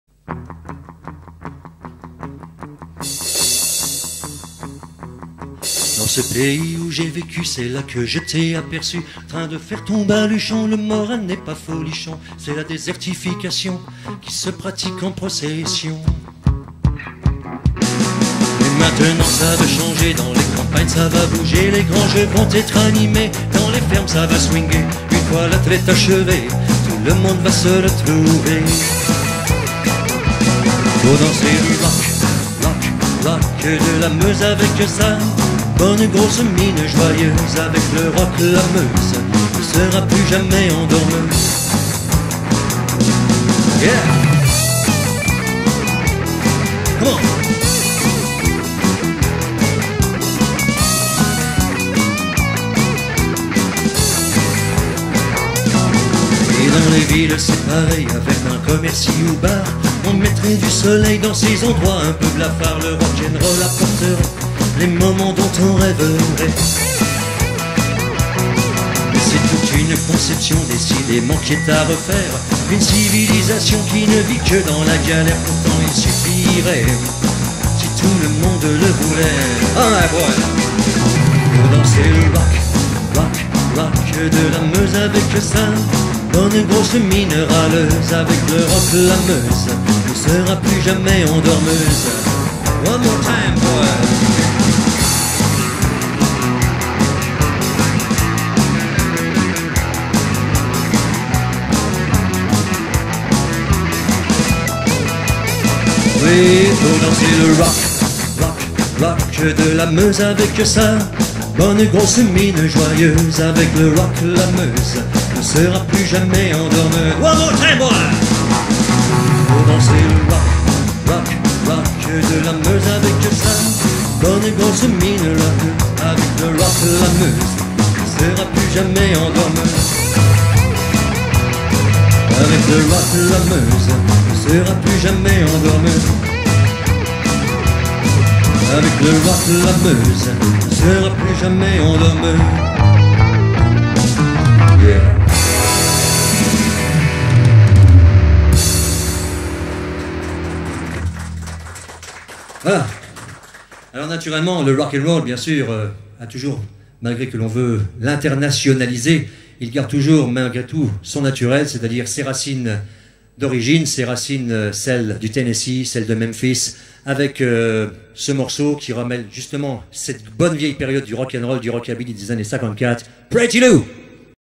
JO GUIHARMO 2014 11 09 09
Captation de l'émission conjointe consacrée à Jo et Cyril donnée en direct depuis le plateau TV de dimanche 9 novembre 2014.